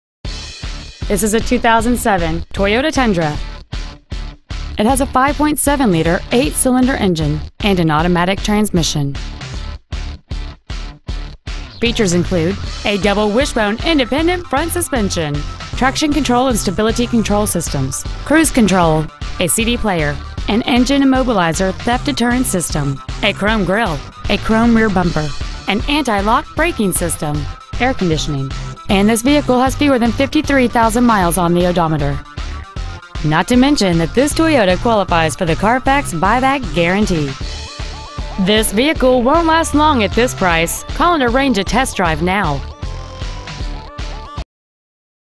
Pre-Owned 2007 Toyota Tundra Grapevine TX 76051
Year : 2007
 Make : Toyota
 Model : Tundra
 Engine : 5.7L DOHC EFI 32-valve V8 engine
 Trans . : Automatic
 Exterior : Gold
 Miles : 52,419
 Interior : Tan

 801 State Hwy. 114 East